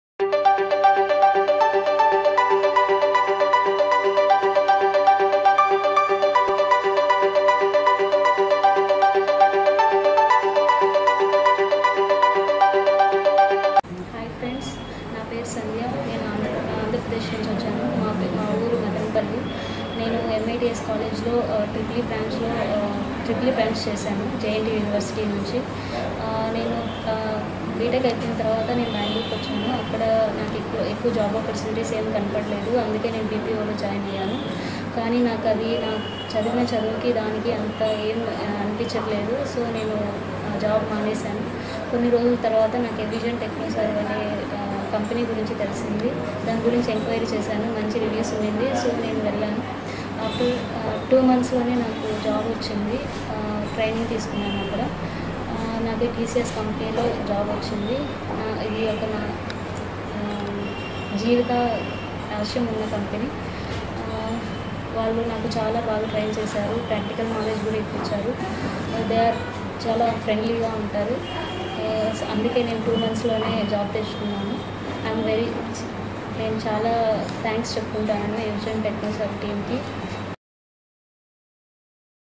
Freshers Recruitment 2020 for Technical Support A Sandhya from Tamil Nadu placed in TCS Telugu
Any Fresher's Graduate Can Apply | Wipro, HCL Technology, IBM, TCS, Jobs 2020 | Fresher's Job

One more happy placement at Evision Technoserve Pvt Ltd when Ms A Sandhya grabbed a job placement in HCL Technology after completed his CCNA and CCNP Job Internship Program from Evision.

Many Many Congratulations to A Sandhya and we wish you all the success in life
Get Training Under Evision Technoserve  Expert

NOTE: PLEASE SHARE RESUME BY WHATS APP TO HR